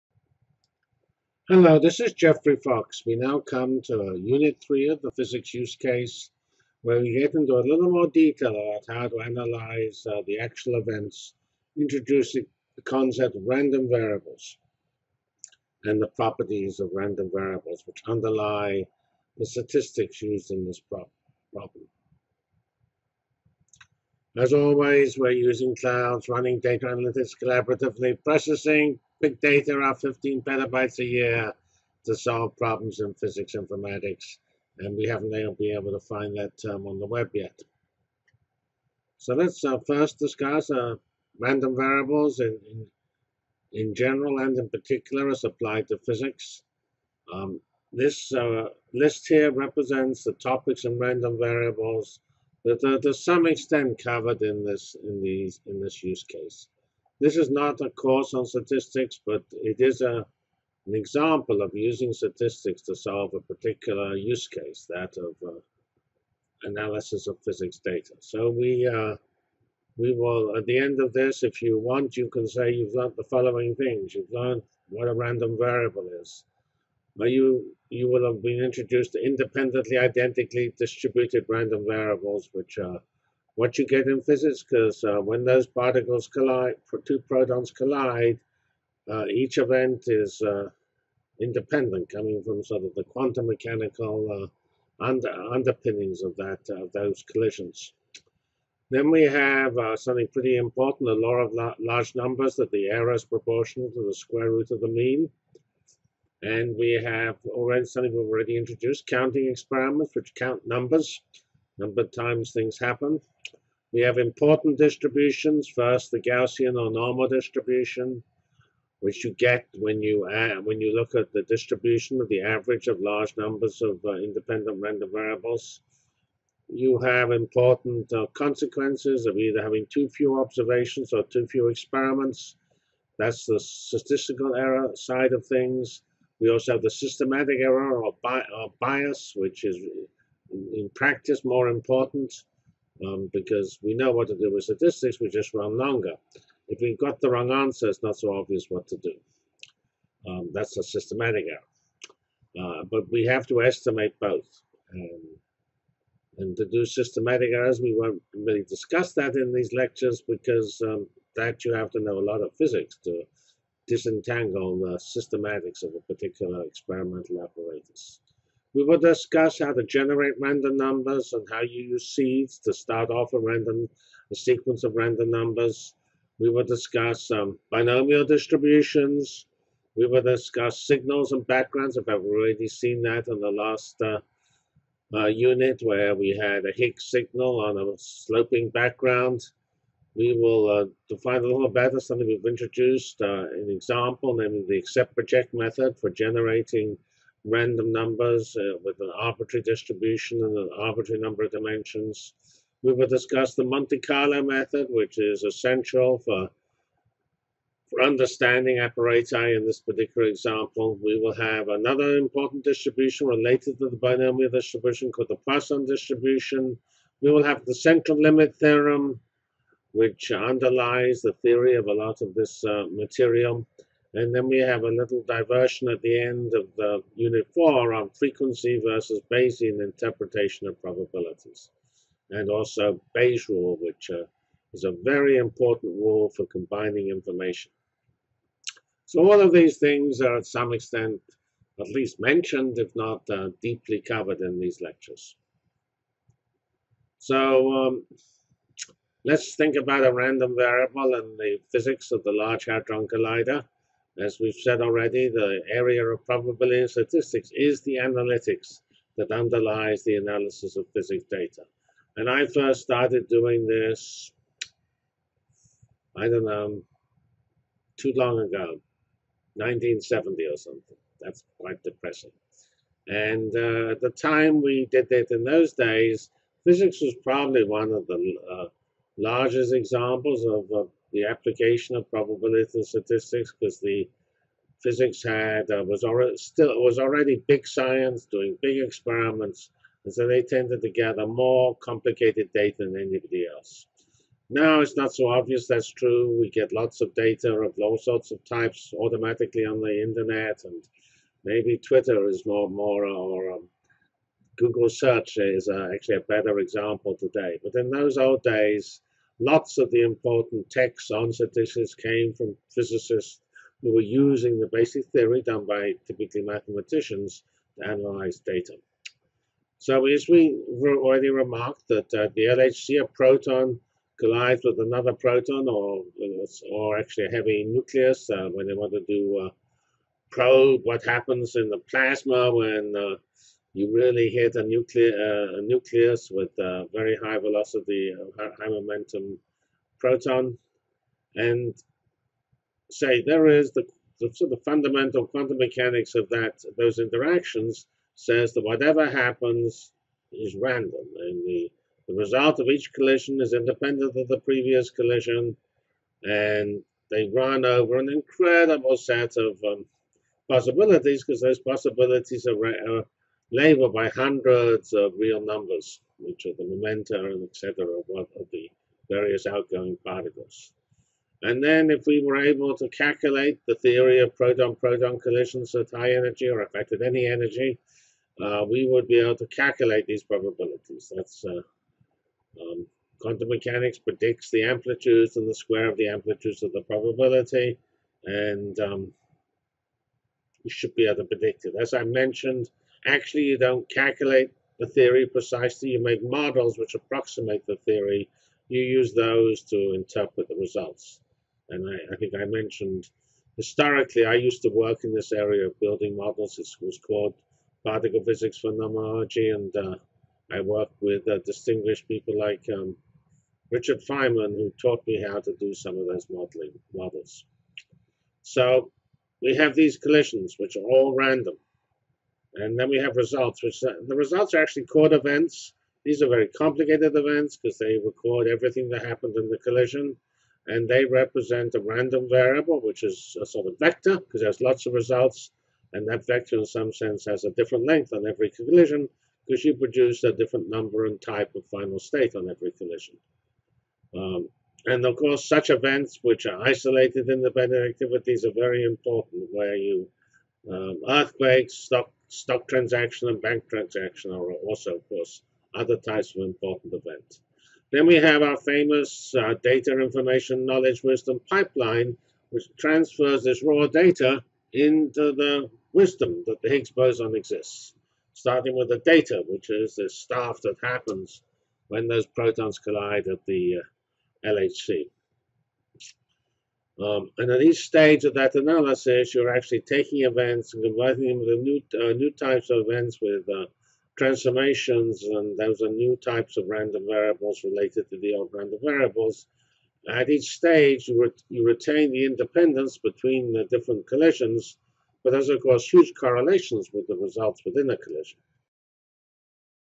Big Data Unit 10: Lesson 1: Statistics Overview and Fundamental Idea: Random Variables
We go through the many different areas of statistics covered in the Physics unit. We define the statistics concept of a random variable